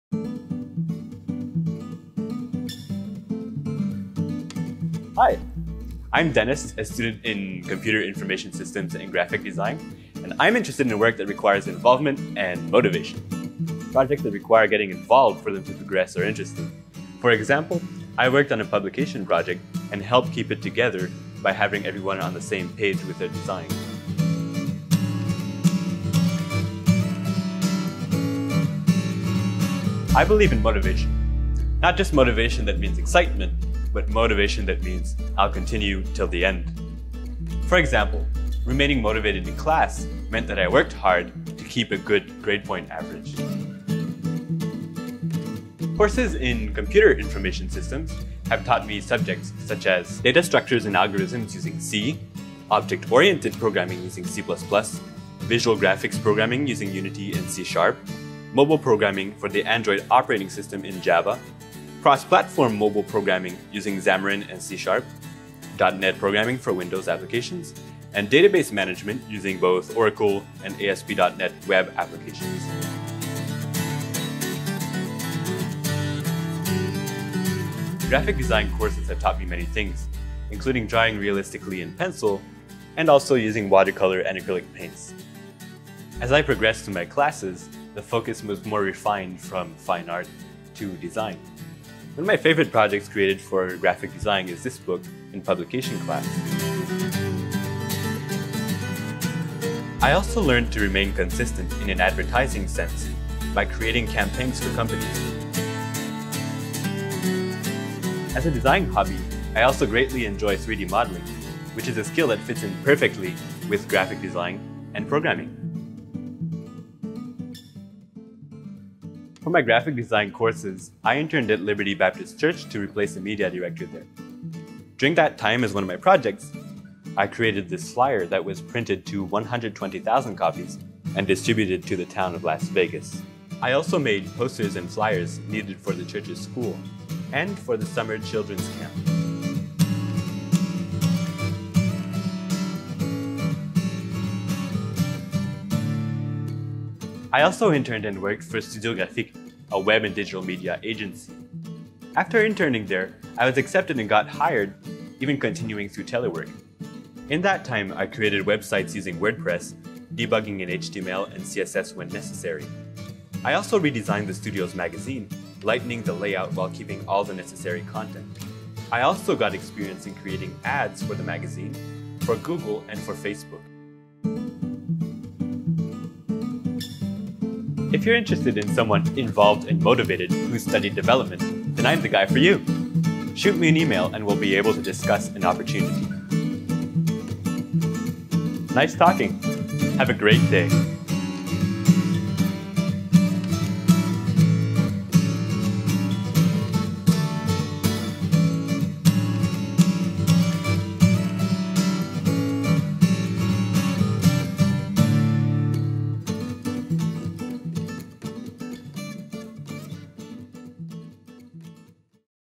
Video Résumé of Programming and Graphic Design Studies
This self-promotional video, made for multimedia class, contains many projects that I made in the last four years at Pensacola Christian College for two bachelor's degrees (Computer Information Systems and Graphic Design).